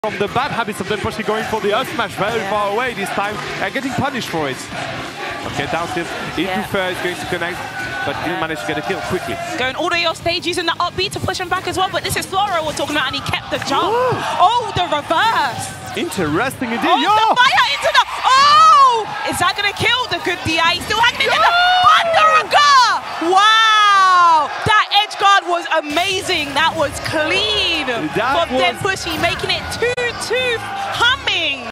Sora Can Edgeguard ANYTHING!
DenPuShi went DEEP for this one! Watch the entire DenPuShi vs kel set from UFA 2023